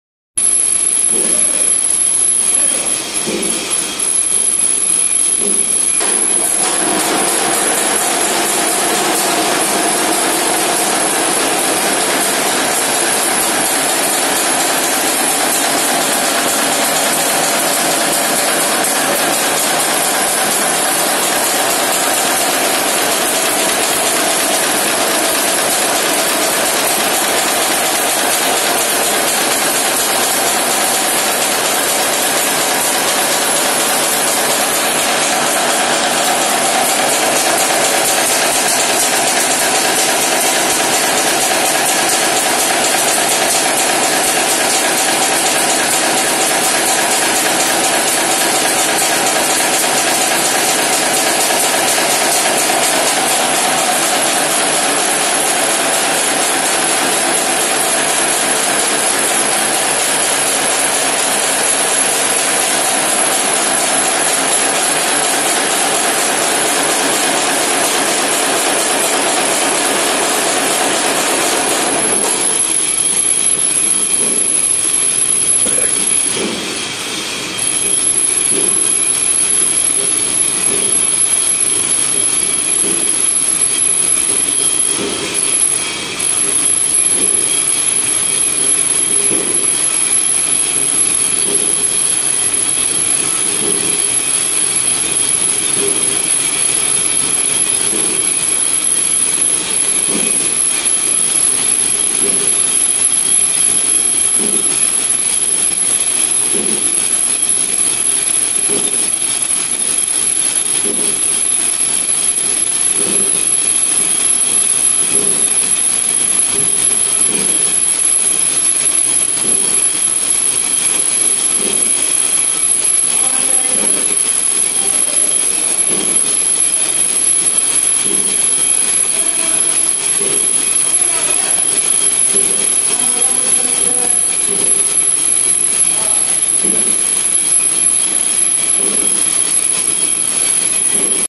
WIRE NAILS MAKING MACHINE
Es machine se aap wire nails ( lohe ki keel ) bana sakte hain har tarah ki har size ki keel machine k kiye call karey ya whatsapp par msg karey

Our some more hydraulic machines